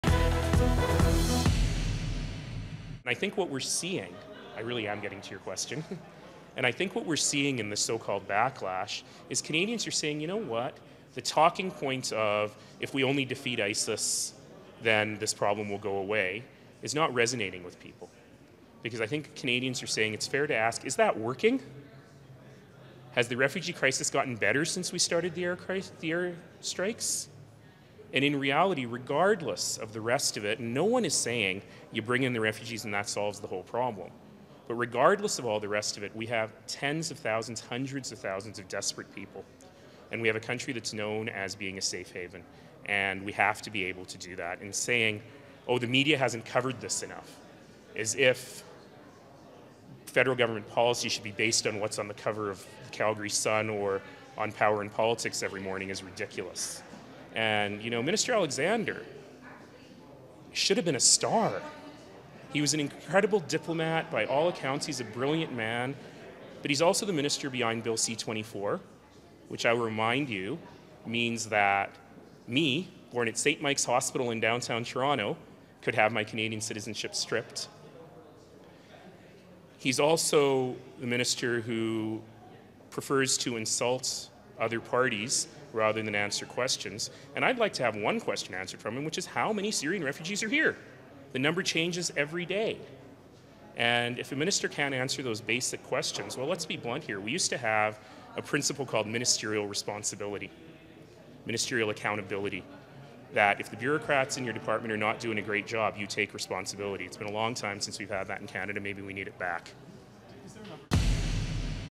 Naheed Nenshi on refugee crisis
Calgary mayor rips into Immigration Minister Chris Alexander on the refugee crisis